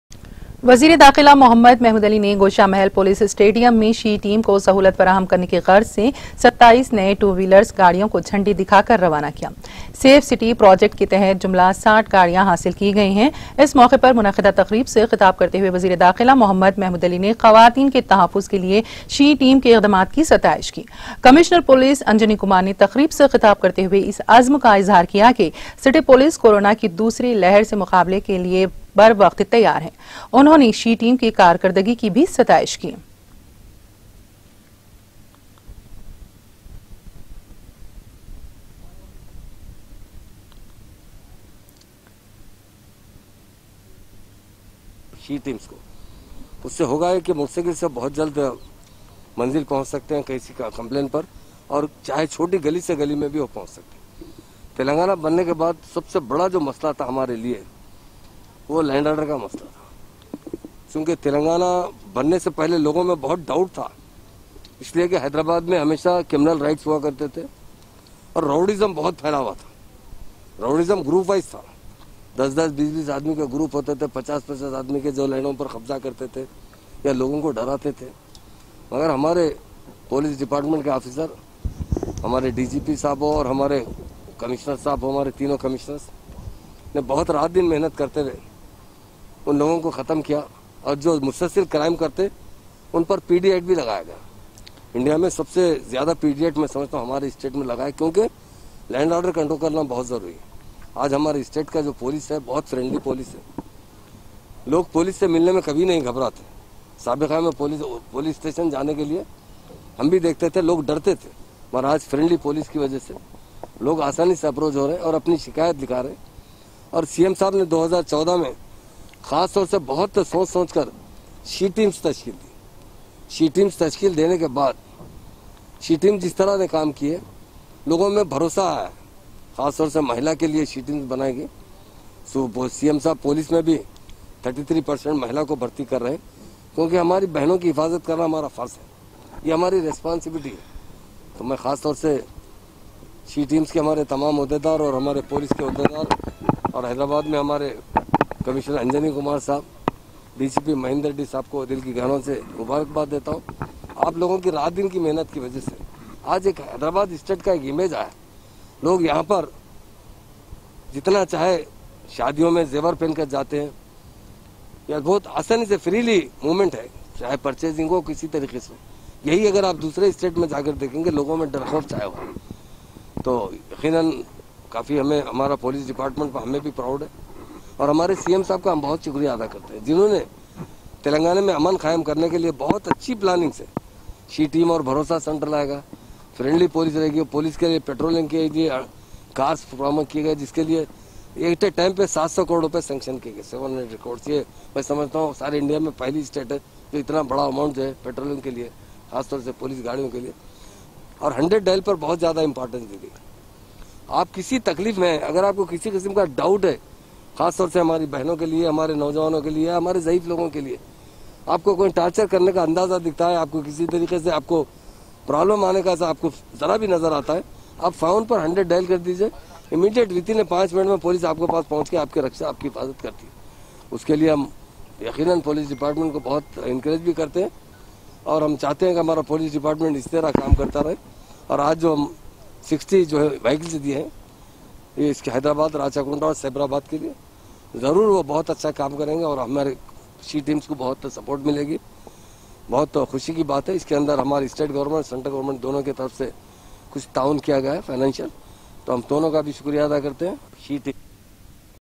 HOME MINISTER MEHMOOD ALI || SHE TEAM || GHOSHAMEHAL POLICE ST || 27 NEW BIKE || T TV NEWS
|| TTV NEWS CHANNEL ||